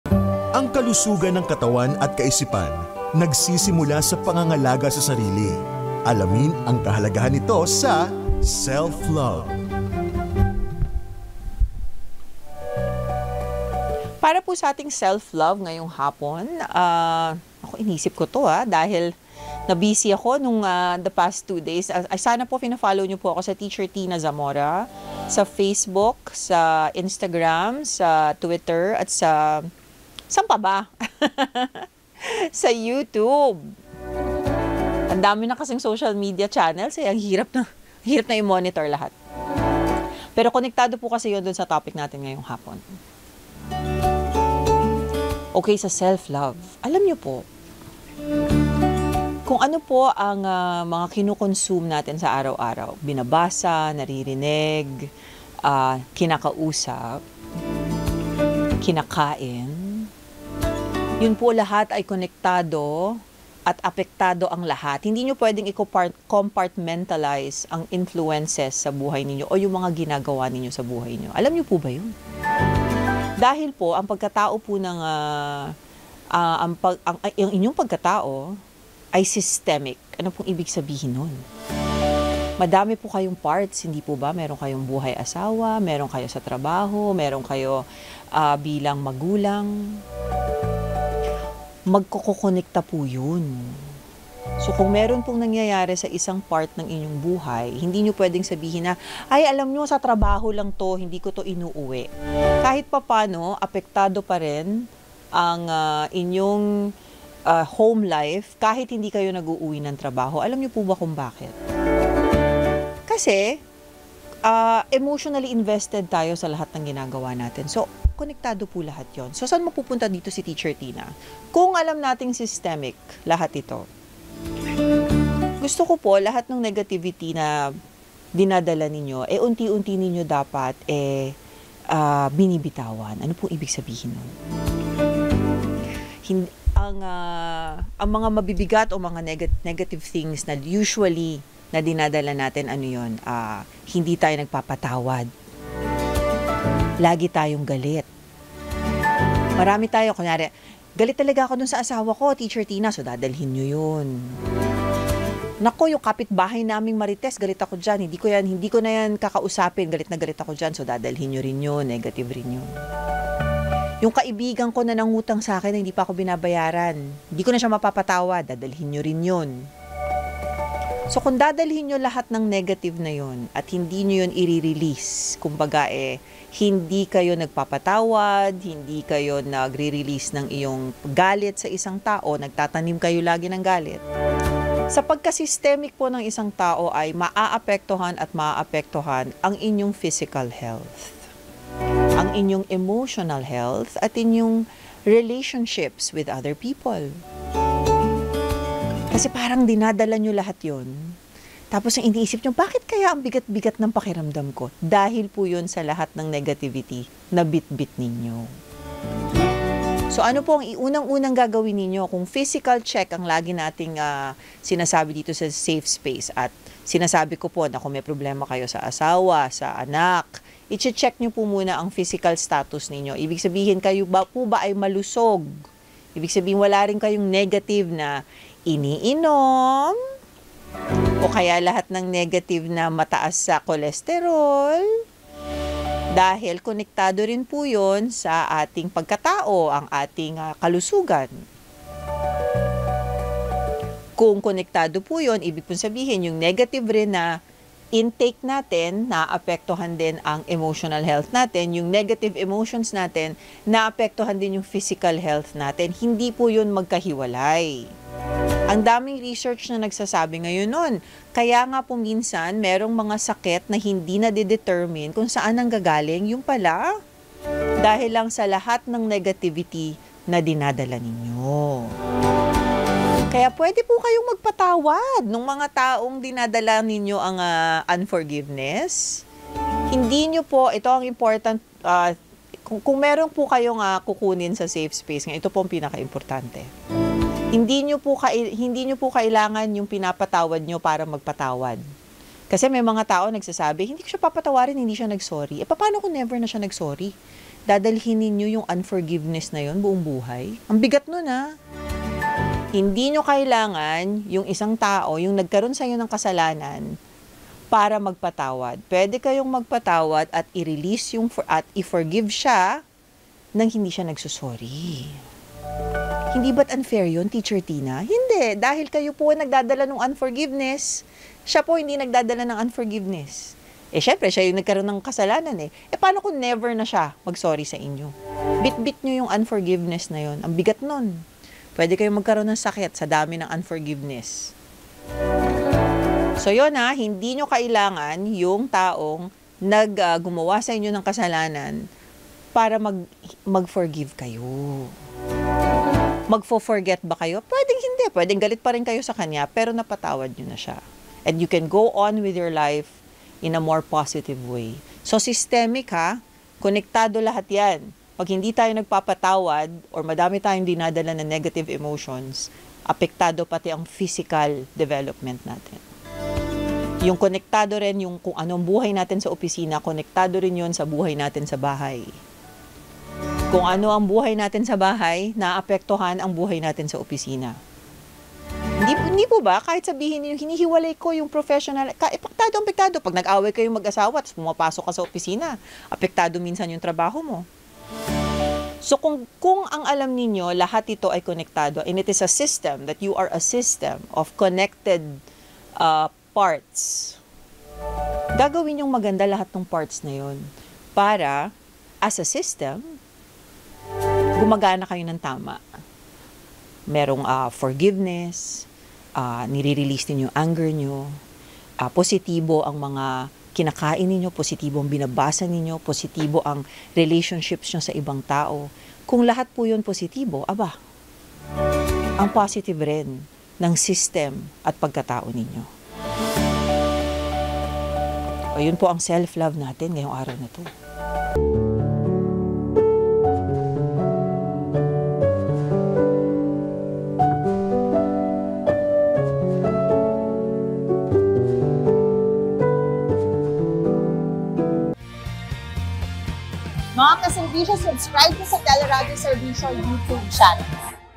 Self-love: Let go of negativities | Safe Space (28 September 2024)
Magpatawad at bitawan ang mga negatibong bagay na iyong dinadala, ‘yan ang self-love ni Teacher Tina Zamora.